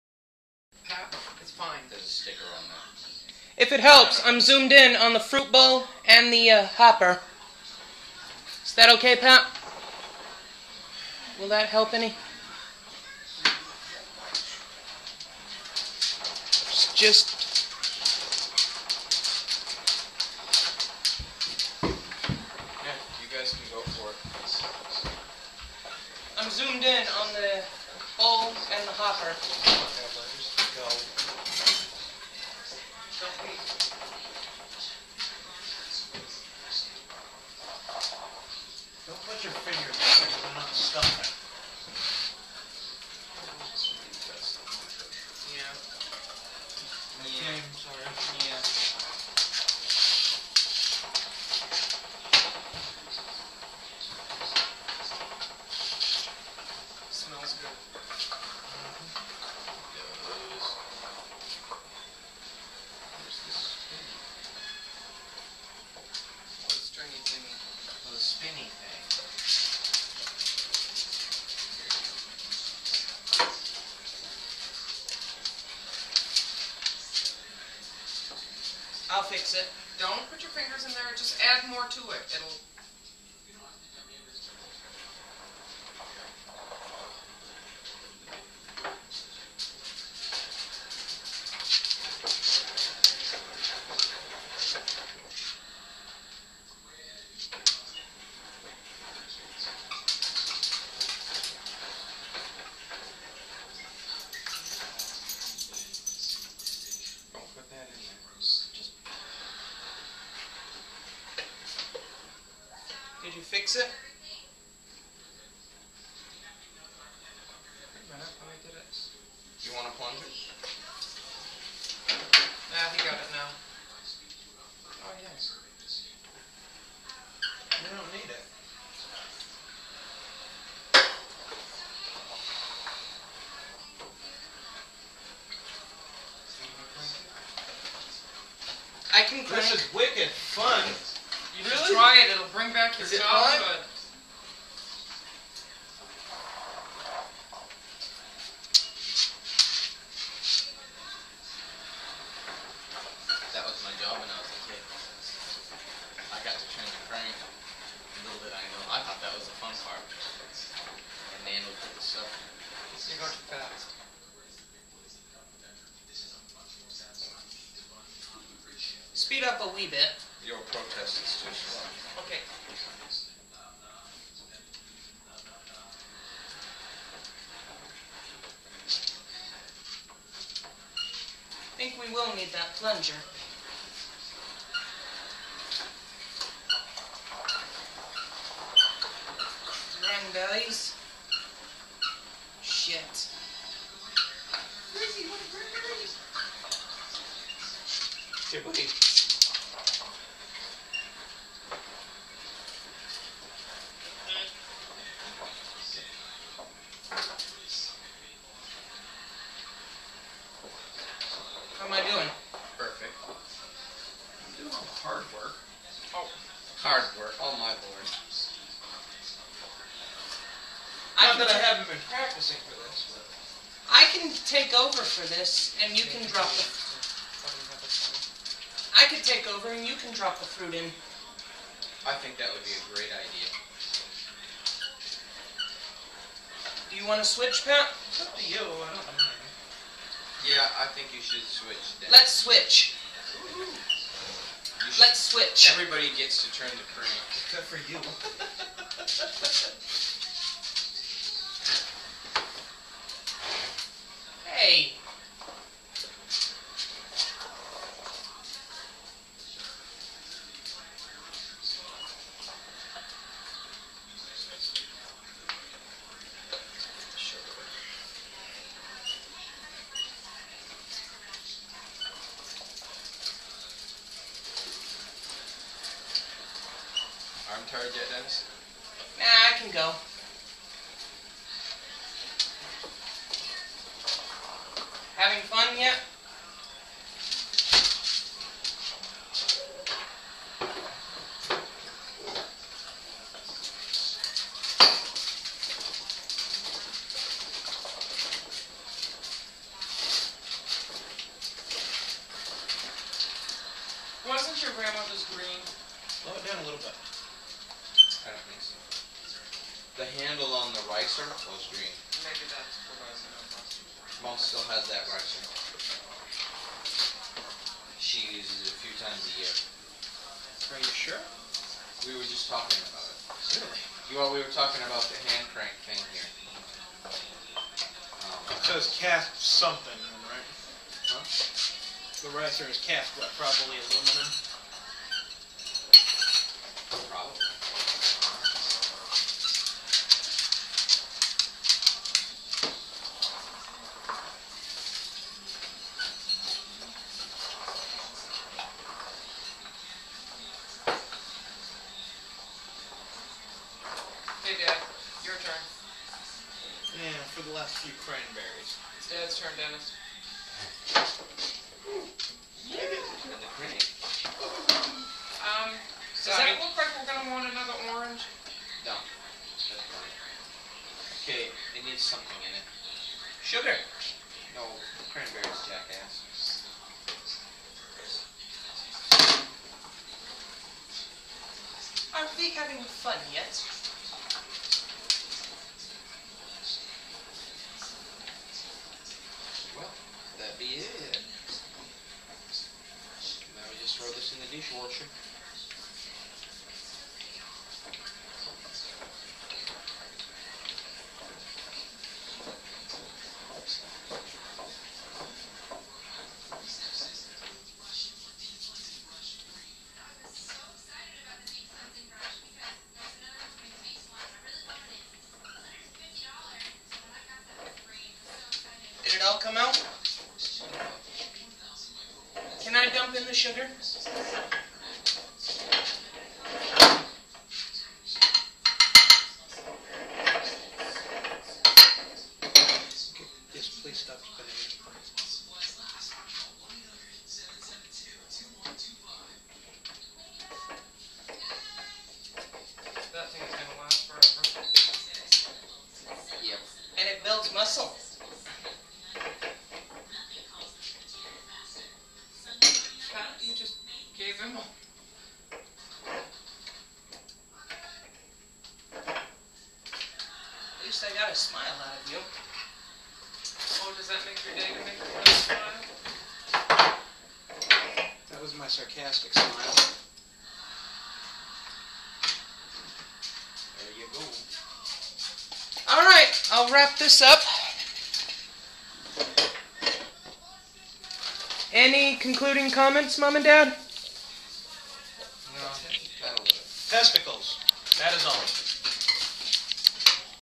Billington Family's Cranberry Relish
This Is Our Favorite Recipe! We Make It Long Before Thanksgiving! To Make It! You Need One Bag Of Cranberries, Two Oranges And Two Gala Apples! You Also Need A Meat Grinder! Work Everything In Nice And Slow! After Everything Is Ground Up, Add One Cup Of Sugar To The Mix! That's All There Is To It! Try This Recipe In Your House! It'll Be Ready By Thanksgiving Dinner!